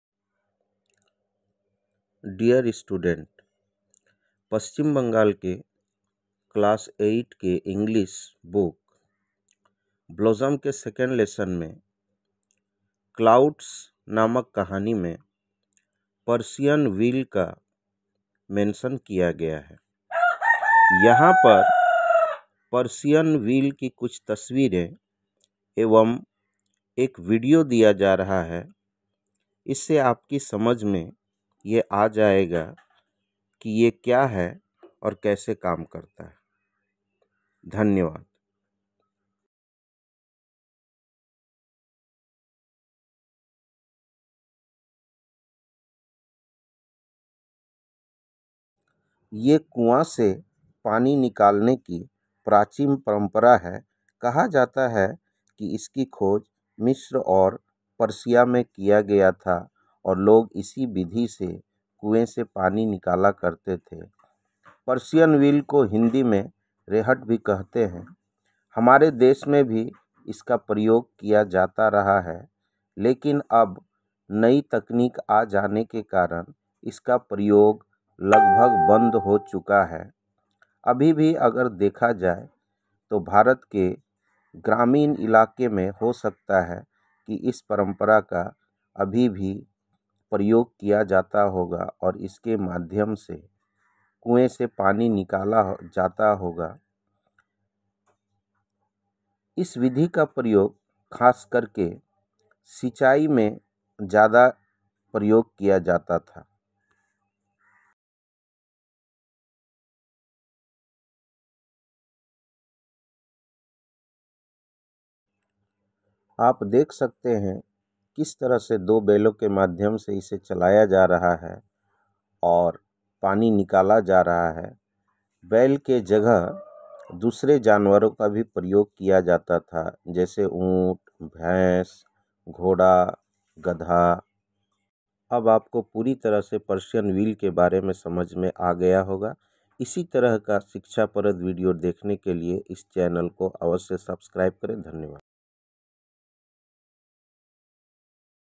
Persian wheel, Blossoms VIII WBBSE, Lesson-2, Clouds..
Subject:- English
Story:- Clouds
Author:- Intizar Hussain
Book:- Blossoms VIII (WBBSE)